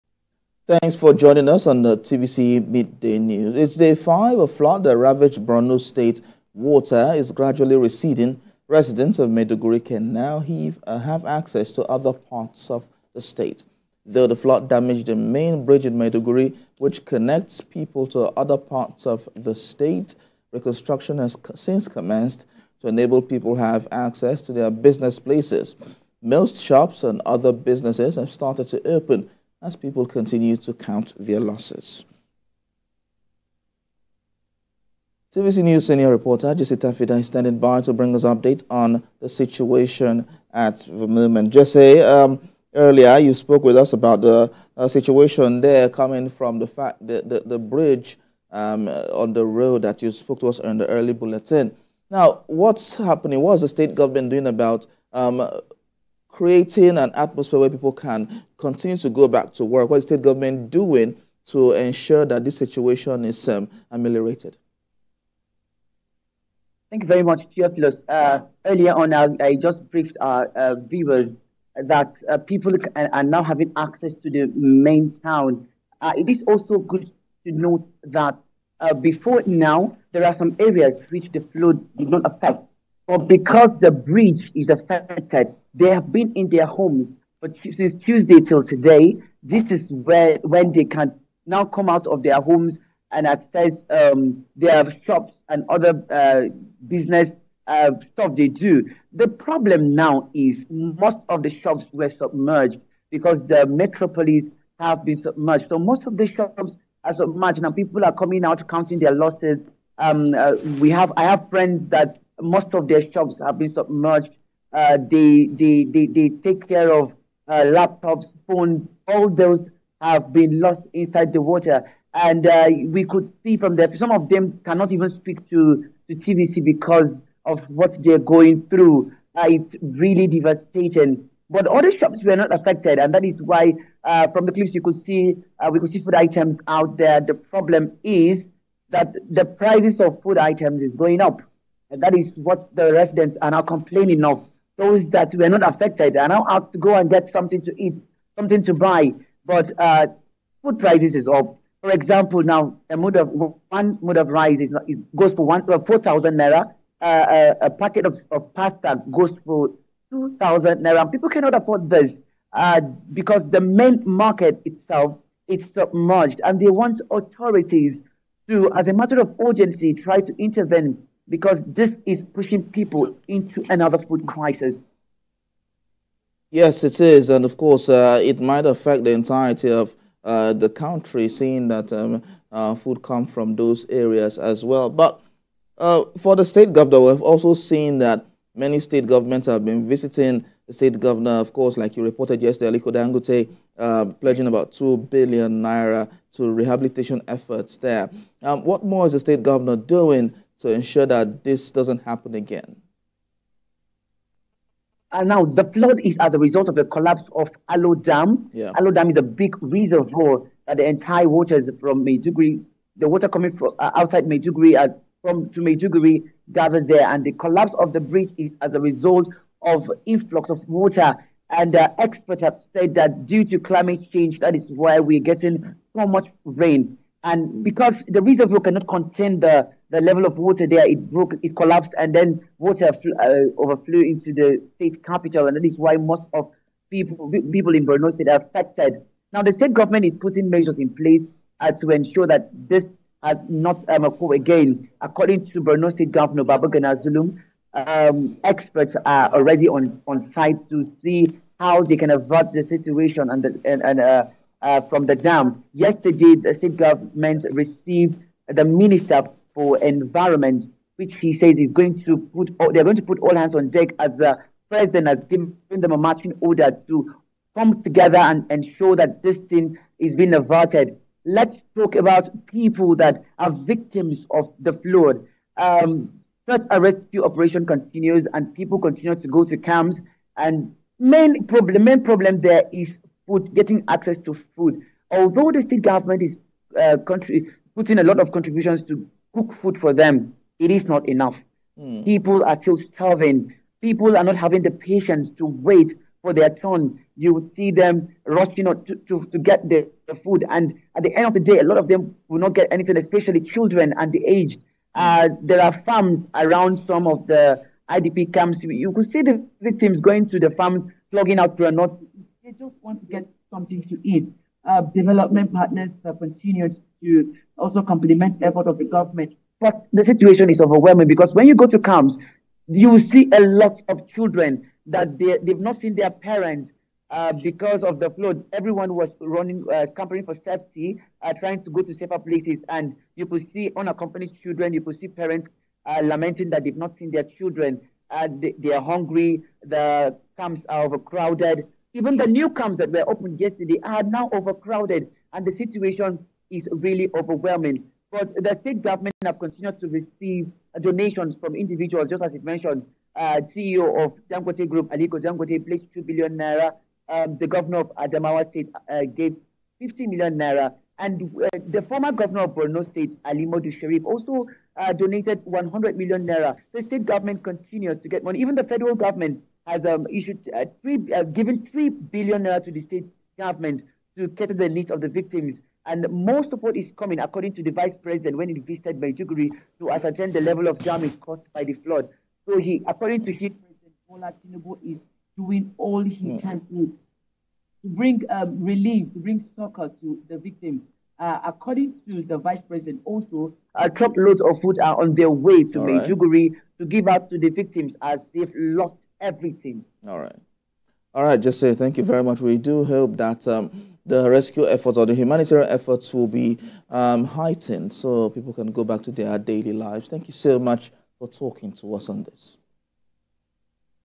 Flood in Maiduguri: Residents Regain Access to Town as Water Gradually Recedes
Its day 5 of flood that ravage Borno state, water is gradually receding, residents of MAIDUGURI can now have access to other parts of the state.
Though the flood damaged the main bridge in Maiduguri which connects people to other parts of the state, reconstruction has since commenced to enable people have access to their business places.
Most shops and other businesses have started to open, as people continue to count their losses.